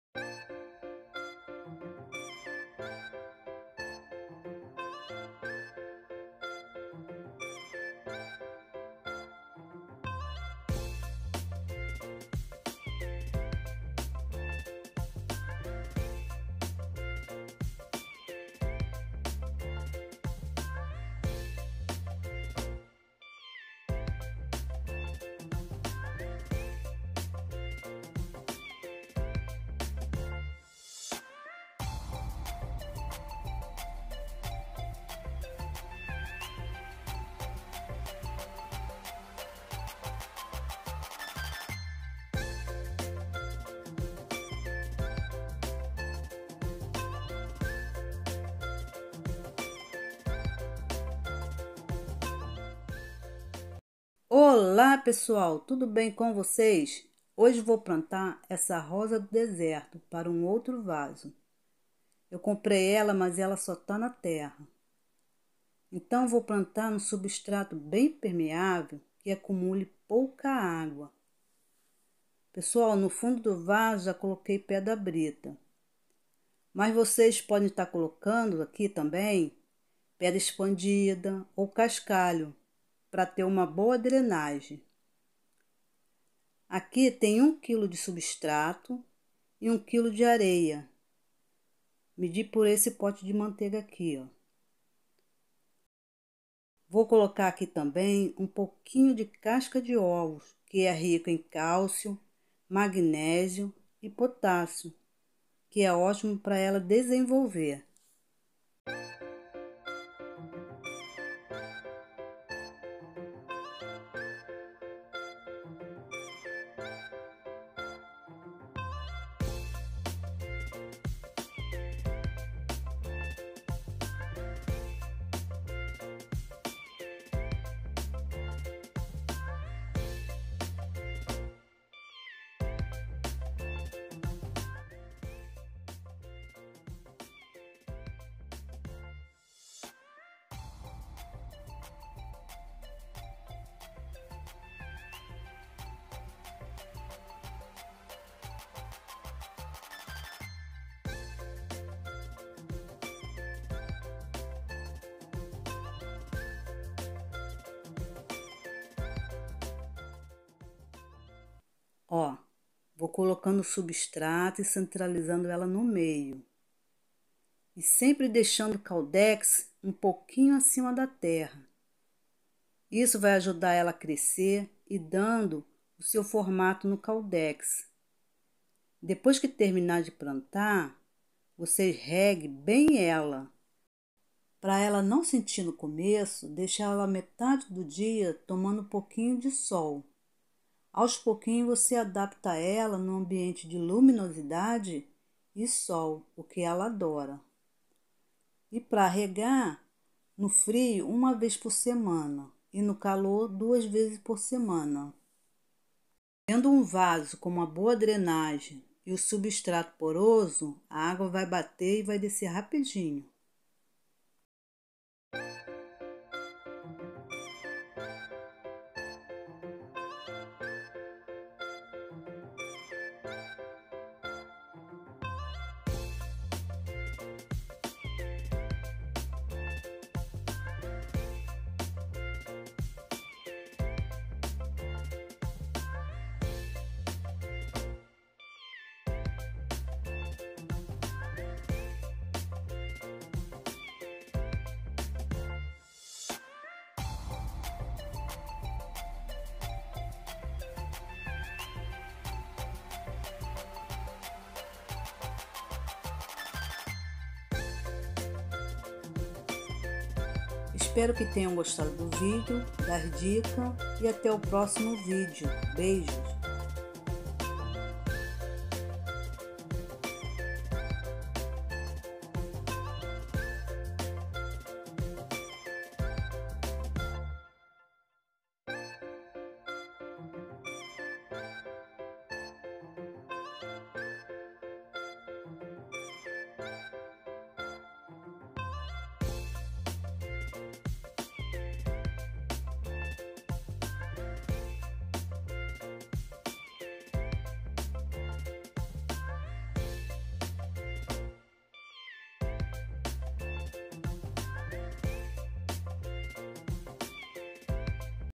PLANTANDO ROSA DO DESERTO .
OLÁ PESSOAL HOJE NESTE VÍDEO VOU PLANTAR UMA ROSA DO DESERTO.
VASO DE 25 CENTÍMETROS DE LARGURA E 15 CENTÍMETROS DE PROFUNDIDADE.
FUROS LARGOS PARA UMA BOA DRENAGEM.
NOS FUROS COLOCAR PEDRA BRITA OU PEDRA EXPANDIDA ,OU SOBRAS DE CASCALHOS .A MEDIDA DA TERRA E AREIA FORAM MEDIDOS PELO POTE DE MANTEIGA DE 1 QUILO .
1 QUILO DE SUBSTRATO E 1 QUILO DE AREIA.E UM POUQUINHO DE CASCA DE OVOS TRITURADOS.
CENTRALIZEI A ROSA BEM NO CENTRO DO MEIO DO VASO .DEIXANDO O CAUDEX UM POUCO ACIMA DA TERRA .PARA TER UM DESENVOLVIMENTO MELHOR ,E TER SEU ASPECTO MAIS BONITO.
REGA: FRIO 1 VEZ POR  SEMANA
REGA:CALOR 2 VEZES POR SEMANA
LOCAL DE AMBIENTE,NO COMEÇO DEIXA ELA A METADE DO DIA TOMANDO UM POUQUINHO DE SOL , COLOCAR ELA ONDE BATA LUZ E SOL .
ESPERO QUE TENHAM GOSTADO DO VÍDEO,DAS DICAS E ATÉ O Próximo
 VÍDEO 💜